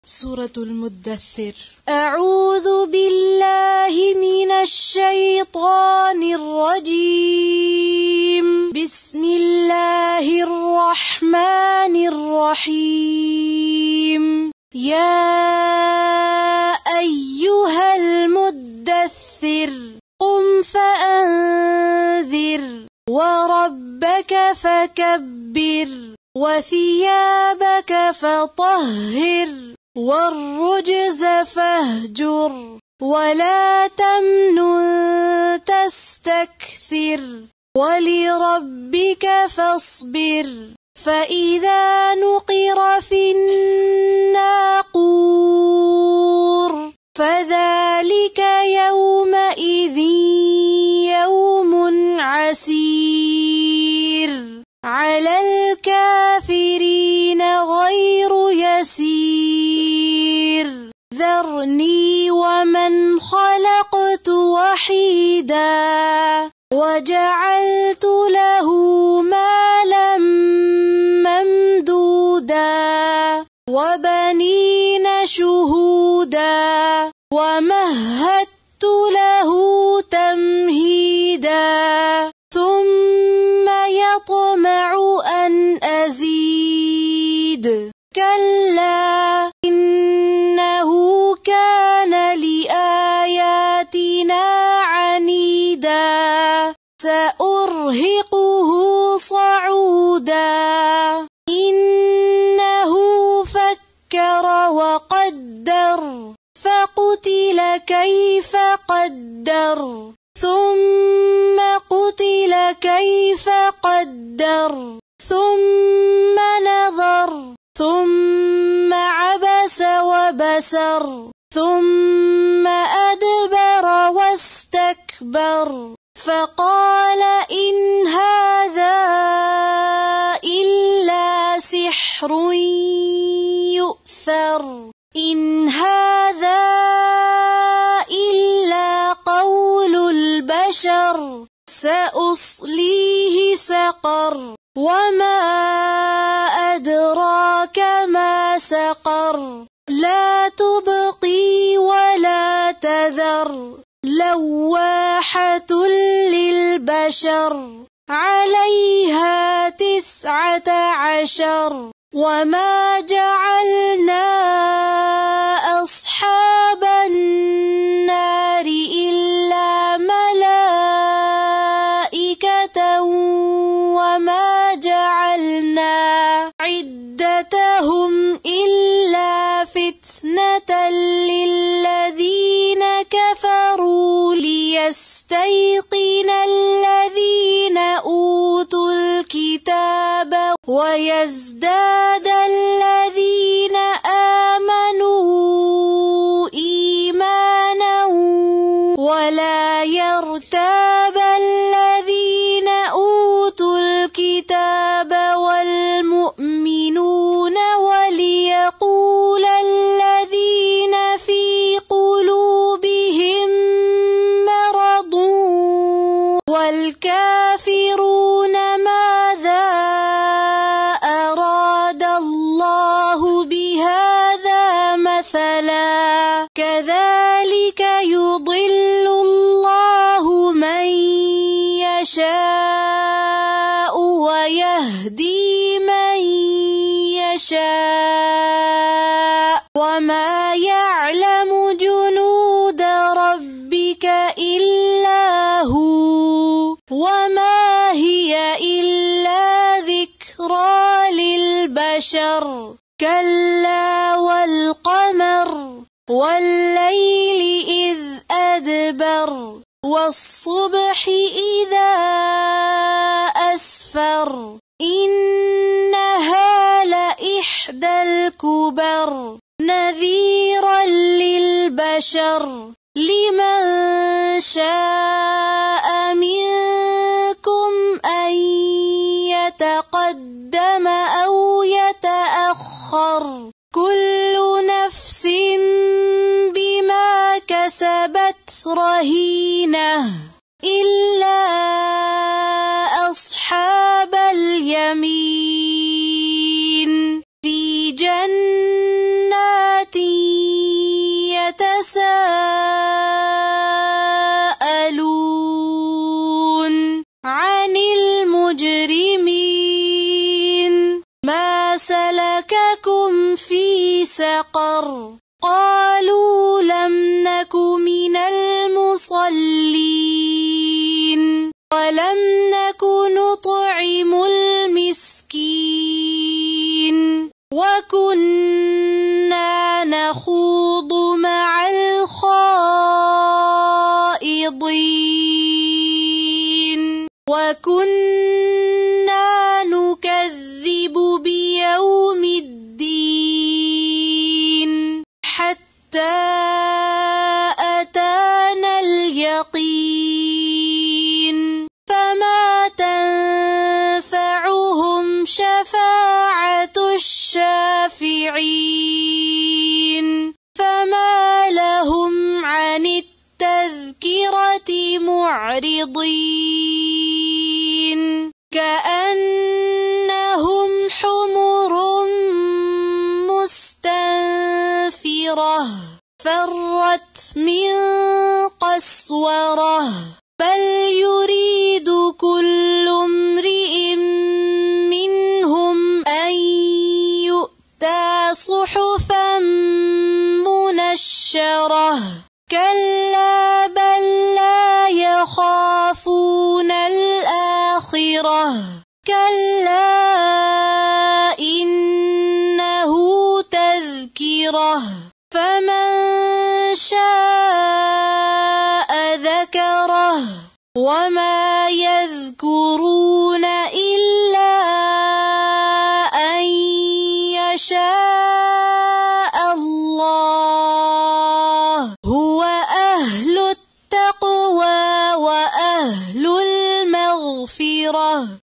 Quran Recitation 74 Surah Al-Muddathhir by Asma Huda with Arabic Text, Translation & Transliteration
Number of Ayaat: 56